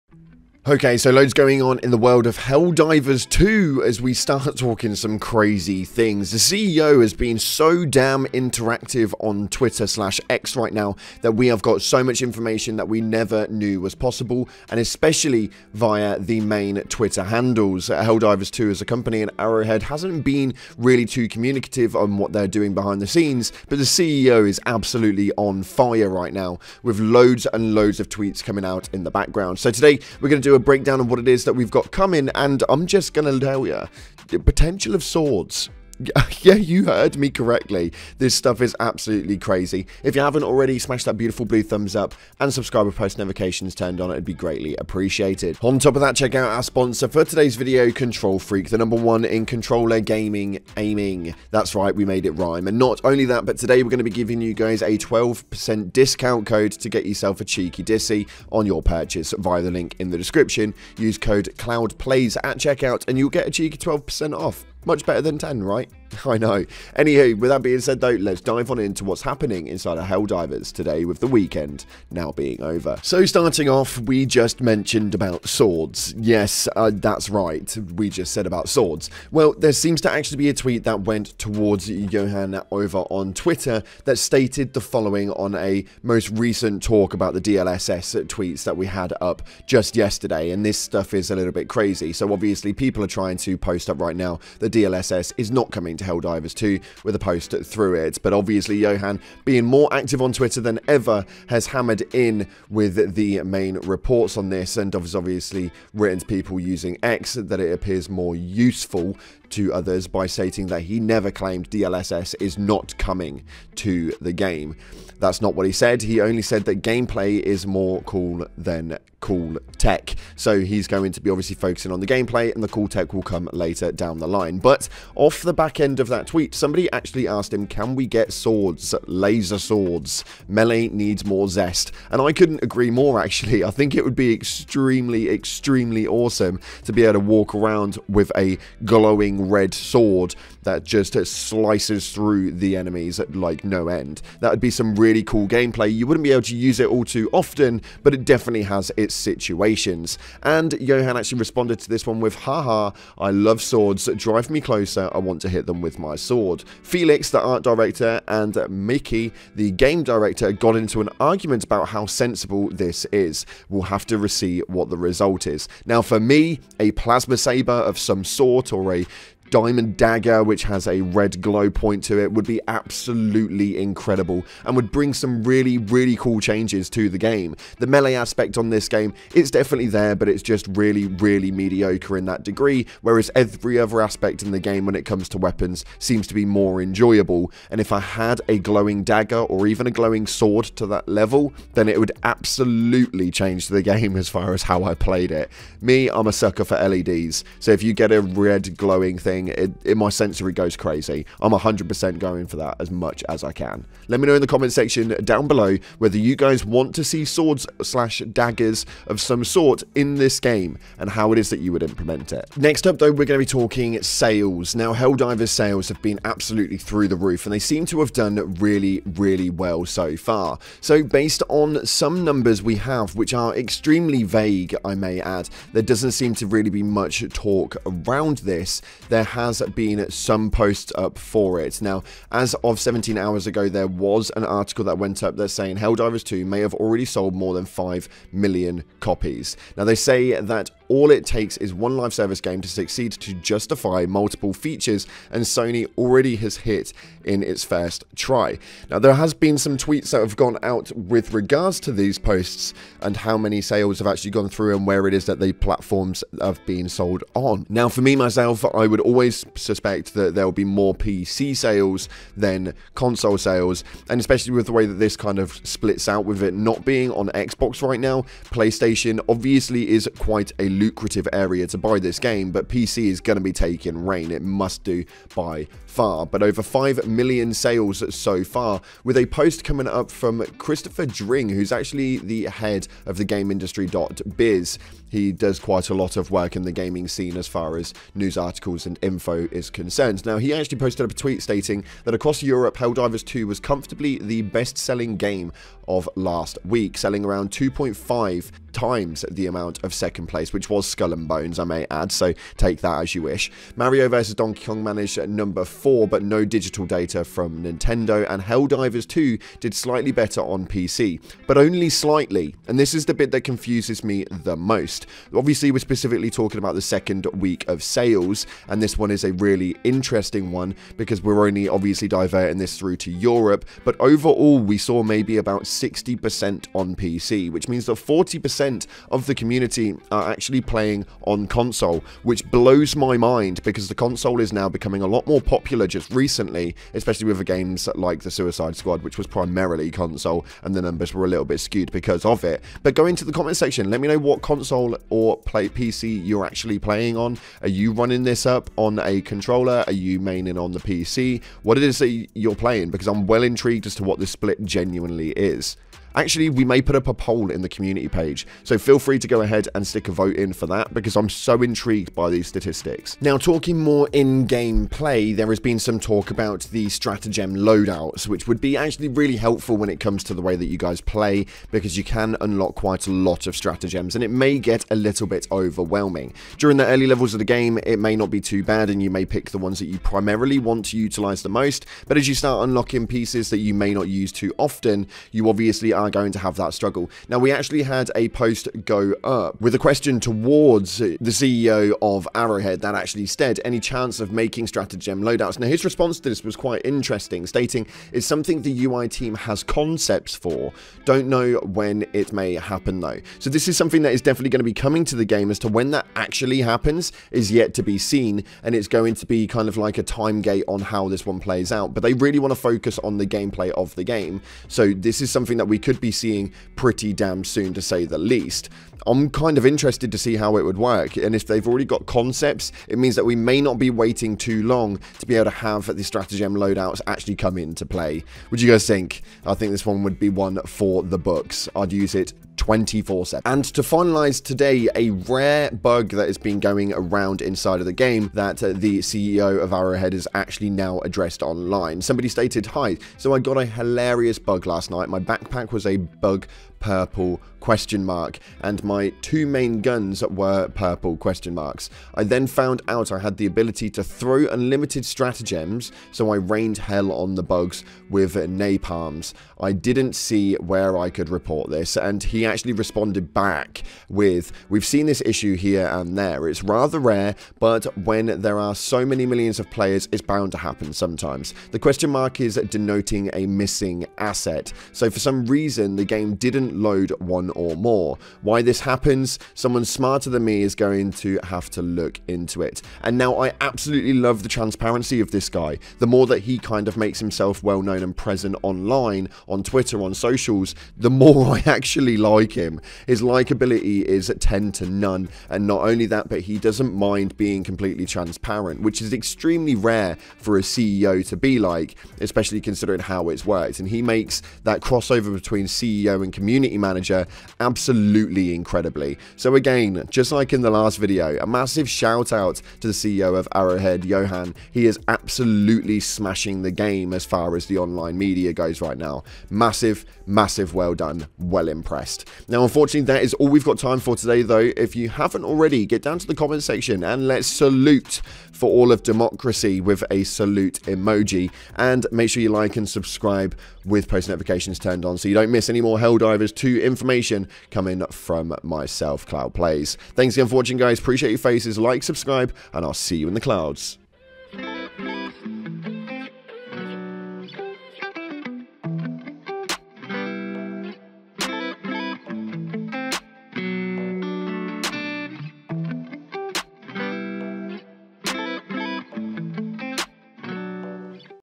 Helldivers 2 Update! CEO Talks About SWORDS In Game! And Over 5 Million Copies Sold!
Helldivers 2 CEO Talks About SWORDS In Game! And Over 5 Million Copies Sold!